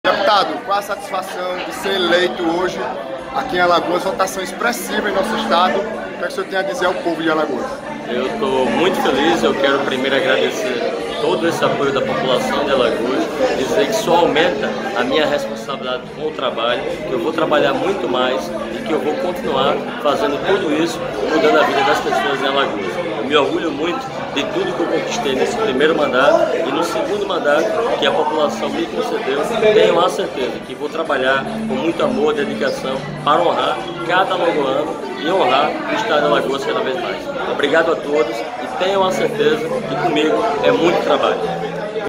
Ao Blog Kléverson Levy, Marx Beltrão explicou os motivos da votação expressiva nas urnas de 2018
Terceiro deputado federal mais votado na eleição do último domingo, 07, o ex-ministro do Turismo e candidato reeleito para o segundo mandato na Câmara dos Deputados, Marx Beltrão (PSD), ganhou 'musculatura' política após a eleição de 2018.